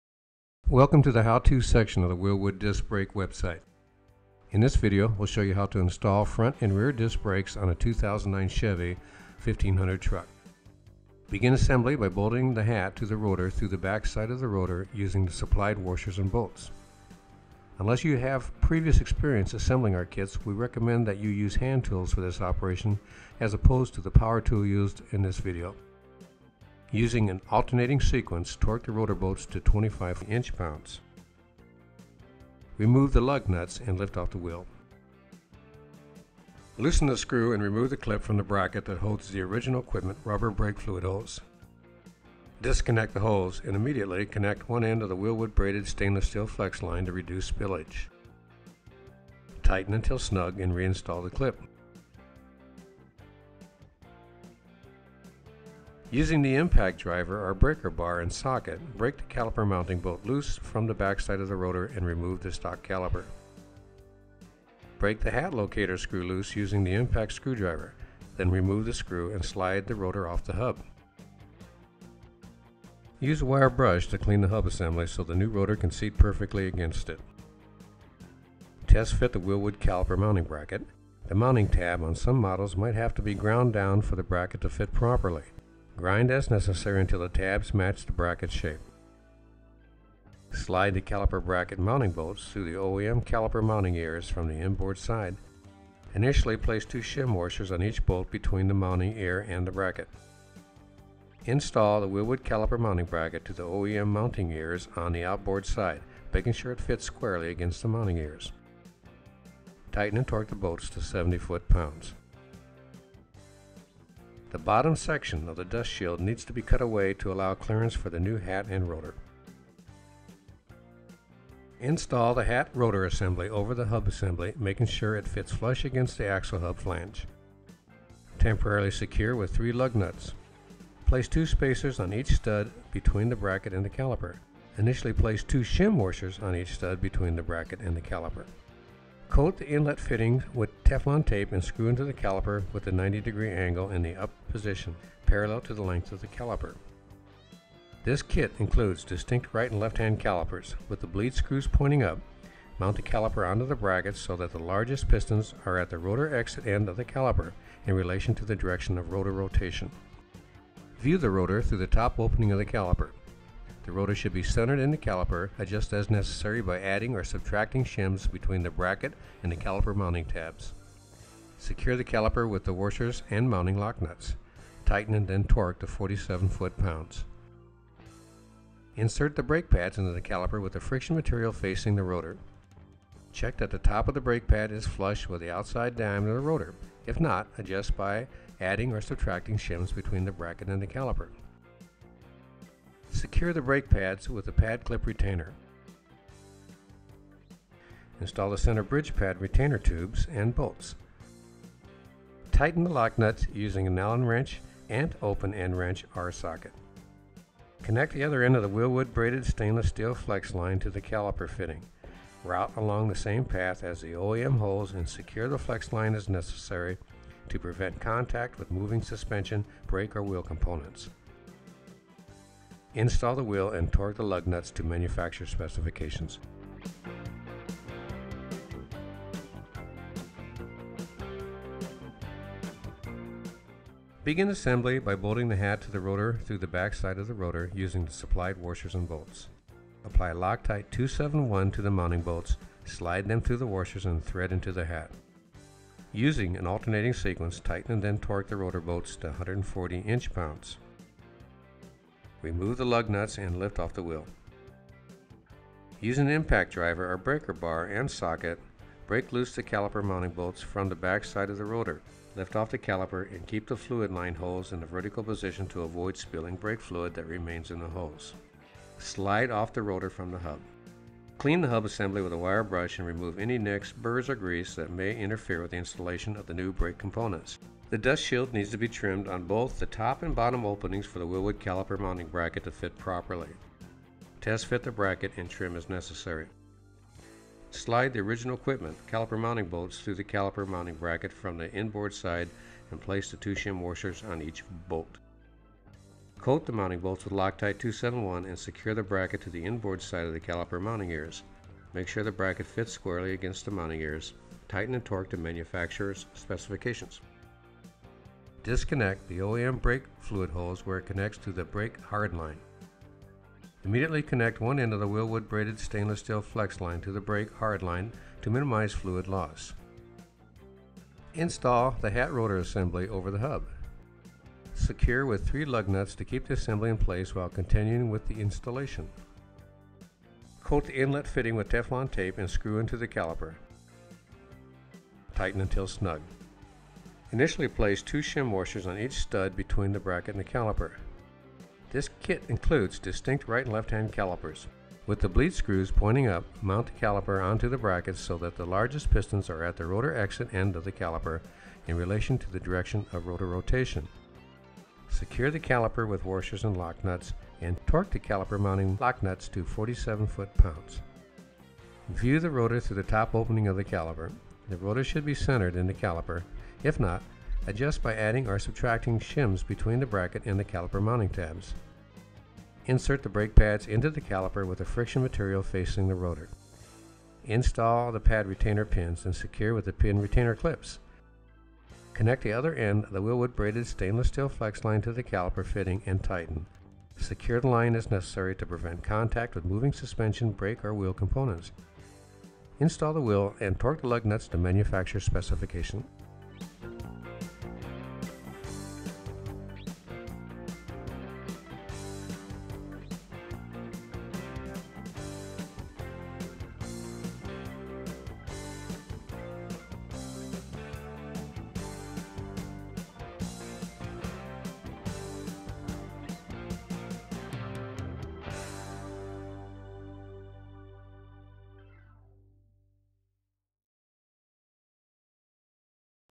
Chevy 1500 Front & Rear Disc Brake Install
Watch as we upgrade this 2009 Chevy 1500 Truck with Wilwood Disc Brakes